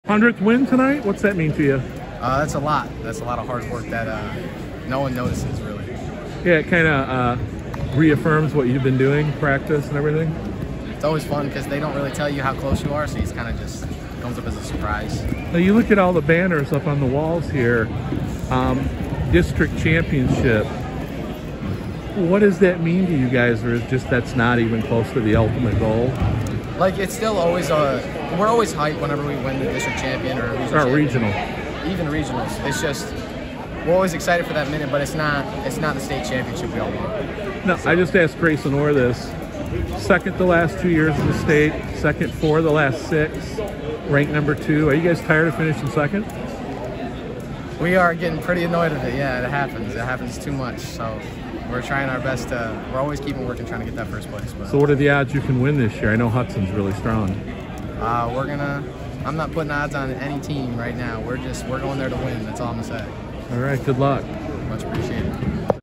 Caleb Sharp wins 100th career wrestling match for New Lothrop
He helped the Hornets win a regional championship